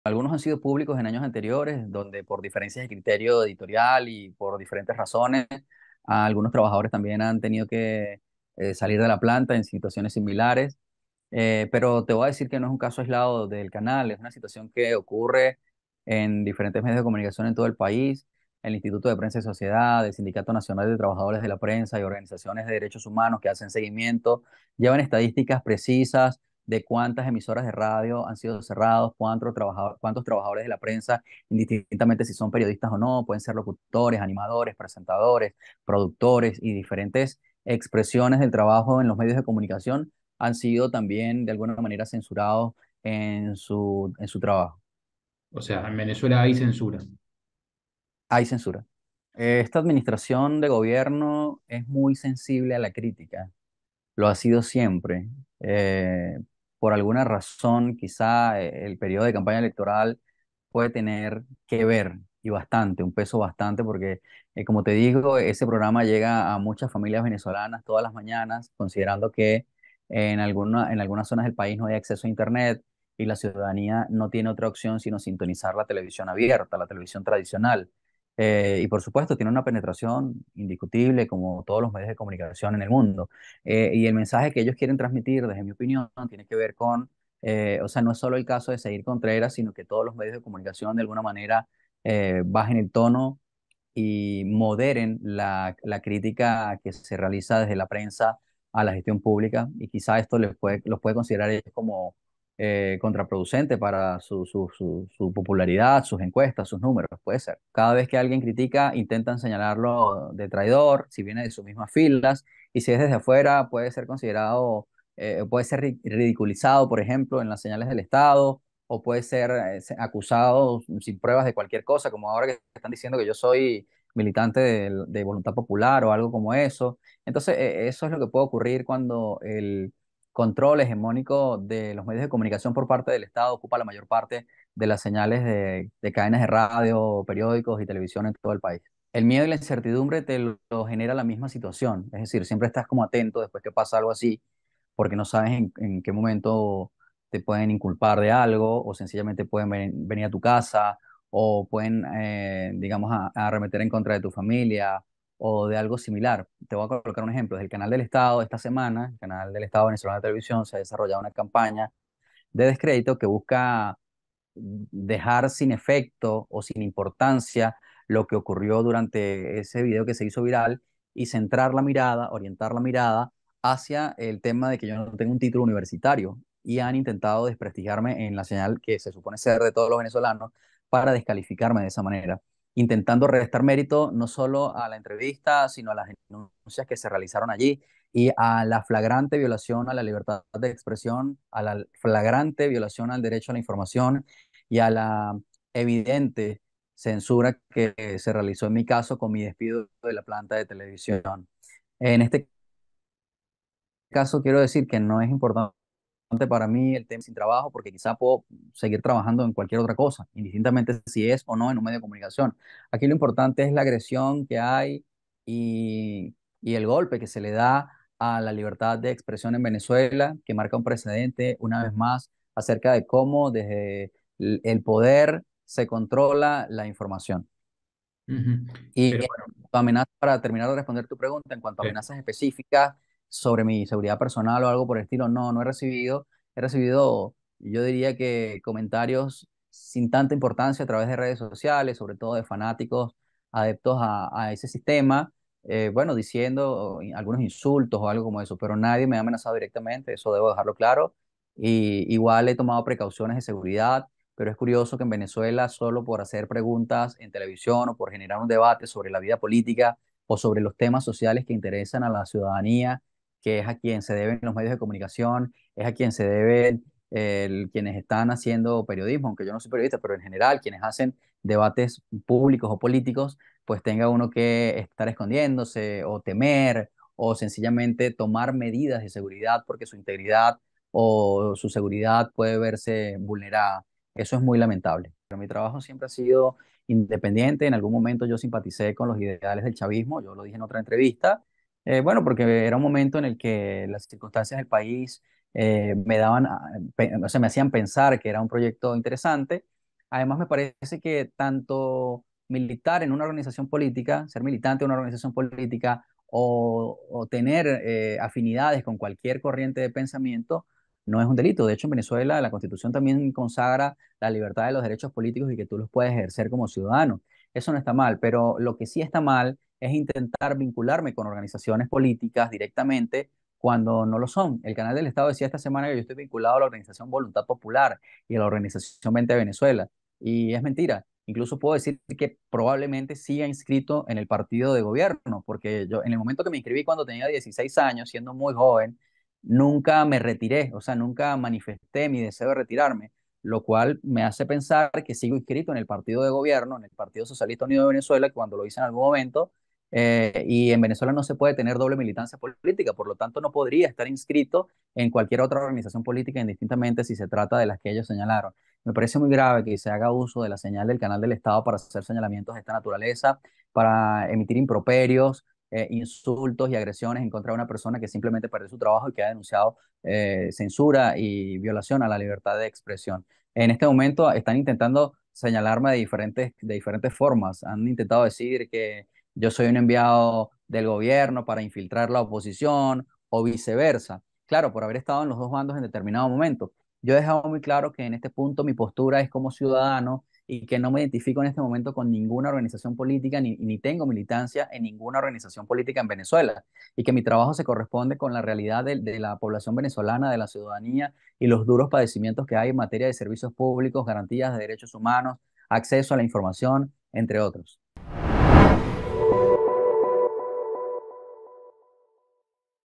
El presentador despedido por debatir con un diputado chavista reveló cómo es trabajar bajo censura
El presentador de TV, Seir Contreras, habló con Infobae tras ser destituido del canal Globovisión, afín al régimen de Maduro. Manifestó que “cada vez que alguien critica intentan señalarlo de traidor”, y reconoció tener “miedo” ante posibles represalias.